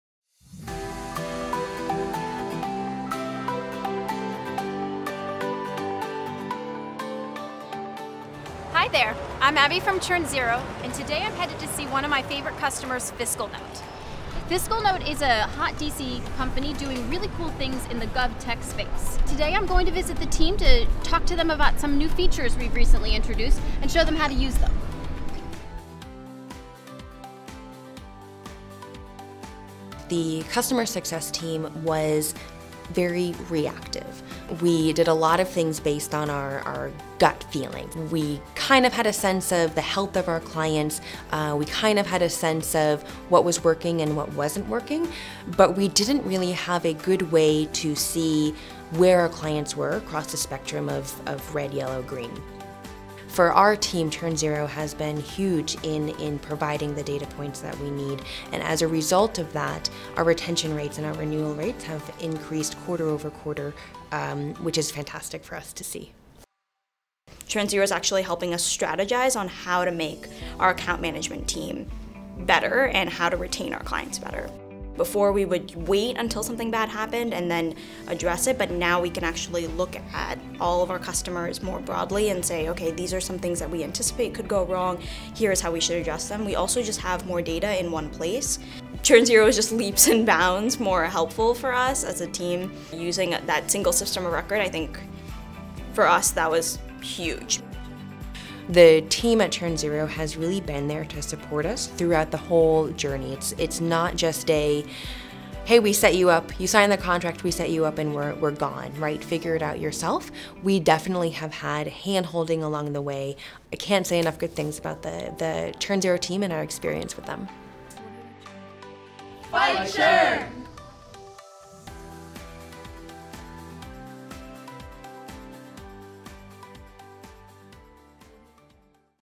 ChurnZero - FiscalNote Customer Testimonial
See how ChurnZero is helping FiscalNote to improve their customer retention quarter over quarter.